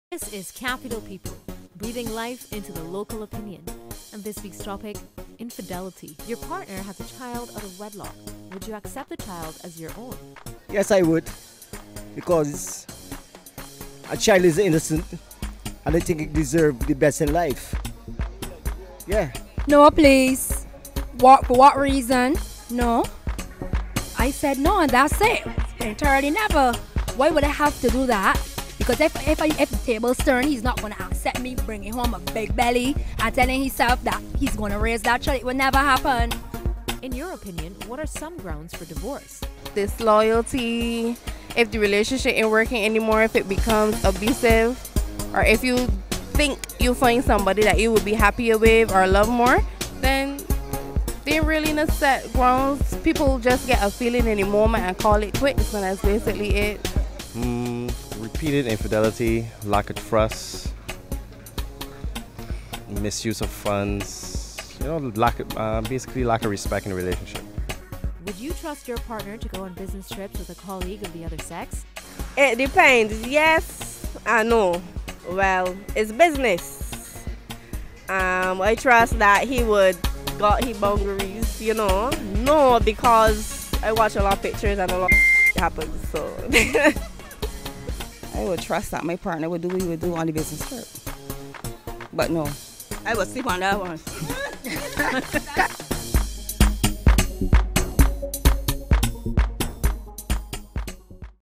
Capital People On The Question of Infidelity
CapitalPeople We took to the streets to breathe life into the people’s opinion on Infidelity and trust!!